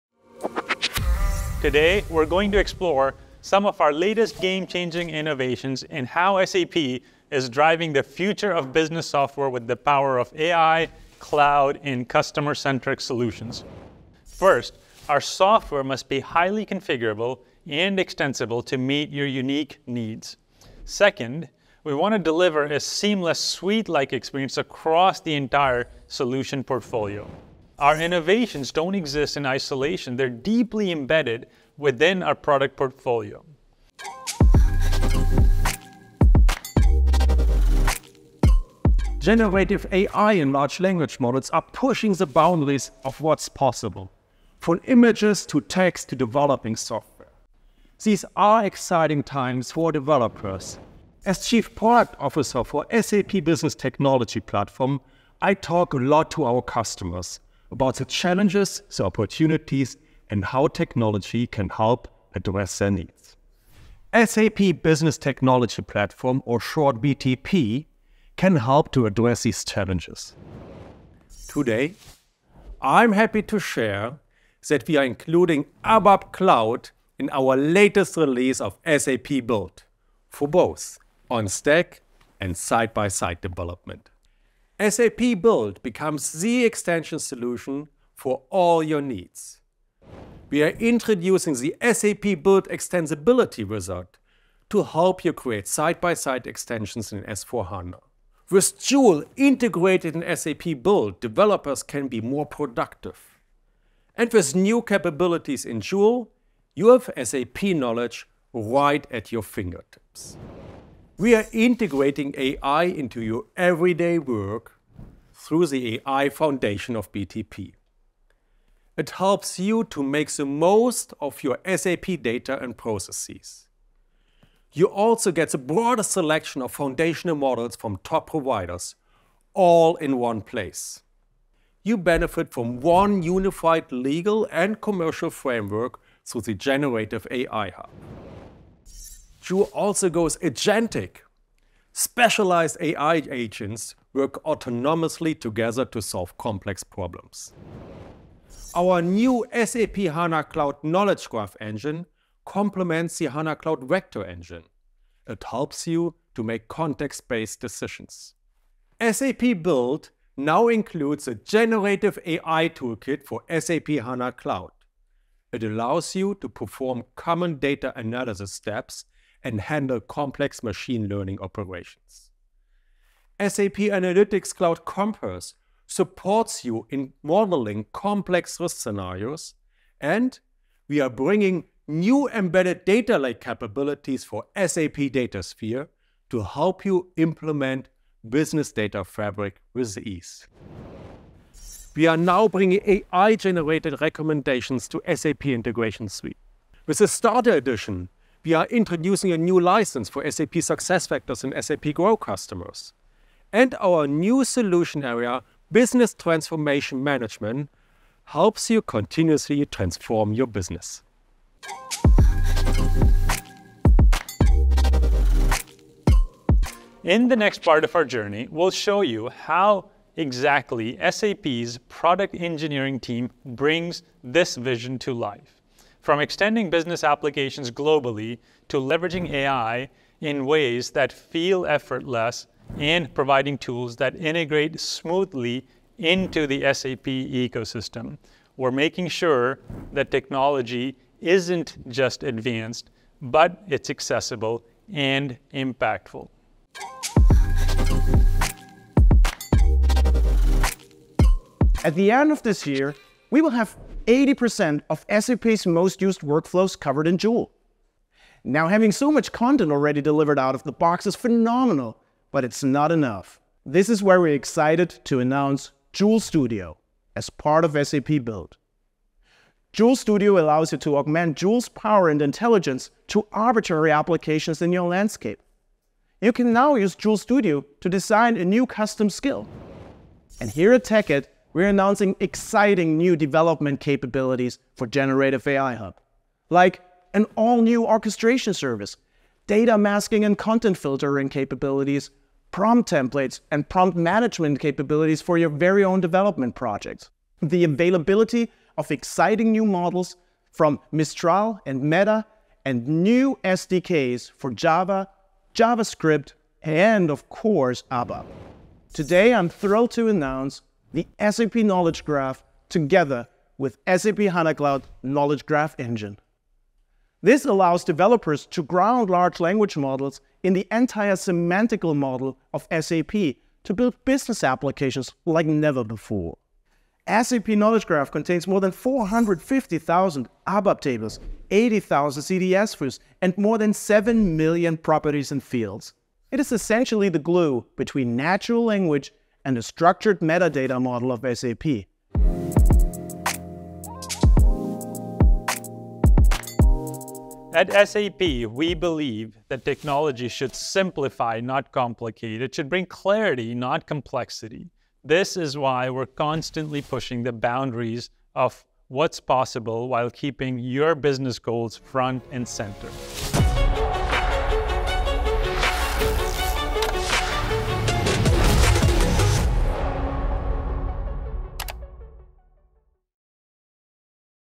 Revolutionizing Tech with AI - SAP TechEd 2024 Keynote Highlights in 6 Minutes
sapteched unveiled the latest innovations that are reshaping the tech landscape. Experience the key moments where SAP Build solutions, SAP Business AI, and SAP Business Technology Platform (BTP) took center stage. Hear how developers, IT professionals, and experts are leveraging these tools to enhance everything from user experience to data-driven decision-making.

00:01 Introduction to SAP's Game-Changing Innovations - Highly Configurable and Extensible Software
00:39 Generative AI and Large Language Models in SAP
01:11 SAP Build and AI Integration
01:51 AI-Driven Business Insights and Automation
03:36 New Capabilities and Tools for Developers

In the next part of our journey, we'll show you how exactly SAP's product engineering team brings this vision to life. From extending business applications globally to leveraging AI in ways that feel effortless, and providing tools that integrate smoothly into the SAP ecosystem - we're making sure that technology isn't just advanced, but it's accessible and impactful. At the end of this year, we will have 80% of SAP's most-used workflows covered in Joule. Now, having so much content already delivered out of the box is phenomenal, but it's not enough. This is why we're excited to announce Joule Studio as part of SAP Build. Joule Studio allows you to augment Joule’s power and intelligence to arbitrary applications in your landscape.